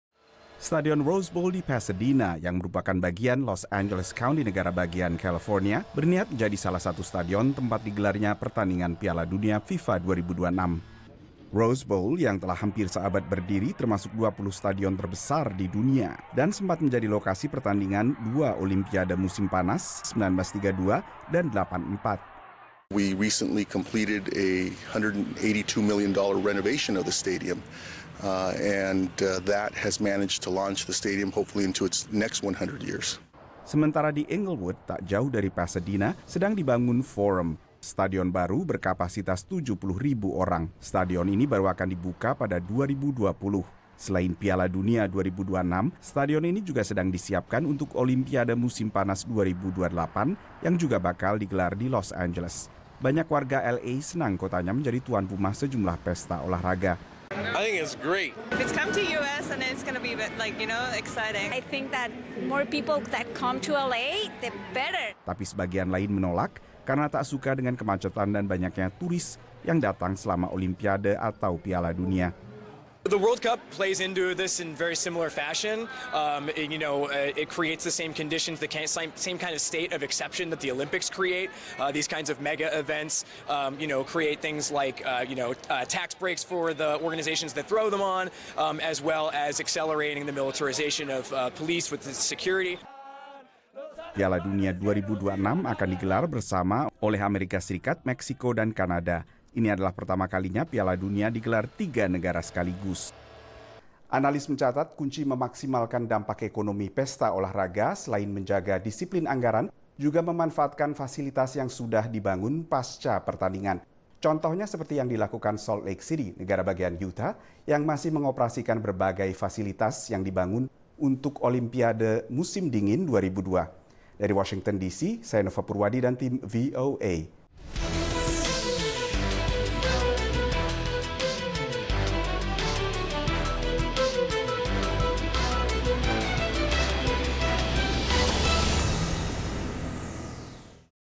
Kompetisi Menjadi Stadion Penyelenggara Pertandingan Piala Dunia 2026
Di tengah demam Piala Dunia FIFA 20018 di Rusia, sebagian penggemar sepakbola di AS juga sudah antusisas menunggu Piala Dunia 2026 yang akan digelar di 16 kota di AS, Kanada, dan Meksiko. 10 di antaranya berada di AS, dan LA dengan berbagai fasilitas olah raganya, berniat menjadi salah satunya.

Caption Foto: Sebuah layar raksasa menampilkan logo Morocco 2026 sebelum konferensi pers untuk mendukung upaya Maroko mencalonkan diri sebagai tuan rumah turnamen sepak bola Piala Dunia 2026, di Casablanca, Maroko, 17 Maret 2018. (Foto:dok) AP